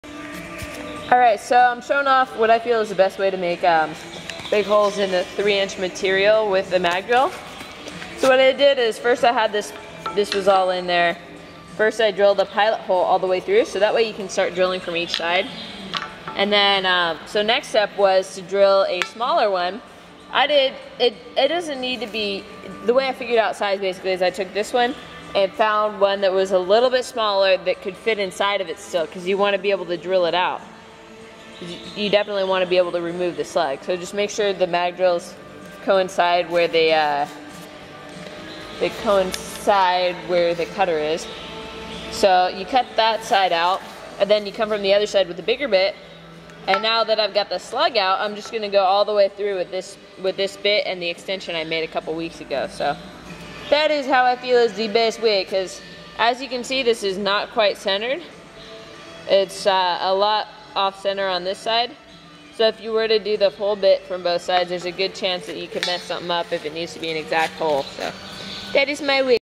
How to make overly deep, but accurate mag-drill holes
First, drill a pilot hole. Then come from one side with a bit slightly smaller than the size you are going for (~1/16-3/16 smaller). Go about half way thru the material. Flip the piece over, and mag-drill with the correct size until it knocks out the slug. Use a mag-drill extension (can make it yourself, see my other vids) to drill thru the remaining material.